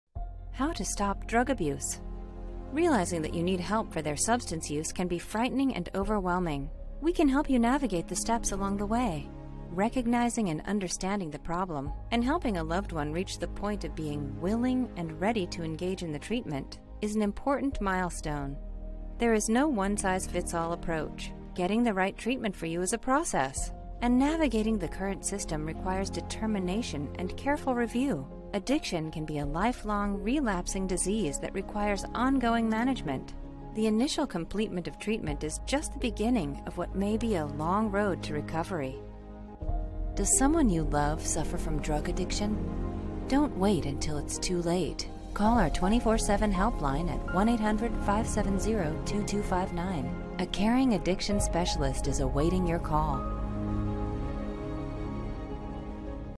How To Stop Drug Abuse?
According to SAMHSA, 40% of adults had a substance abuse disorder. Those who decide to give up drugs and alcohol must go through the various stages of recovery. Realizing that you need help for their substance addiction might be terrifying and intimidating. Recognizing and comprehending the problem, as well as aiding a loved one in becoming willing and ready to participate in treatment, is a key milestone.